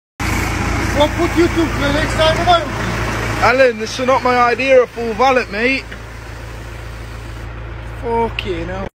Guy Cleaning my Car Used a Pressure Washer on the Inside || ViralHog
I only wanted it cleaned...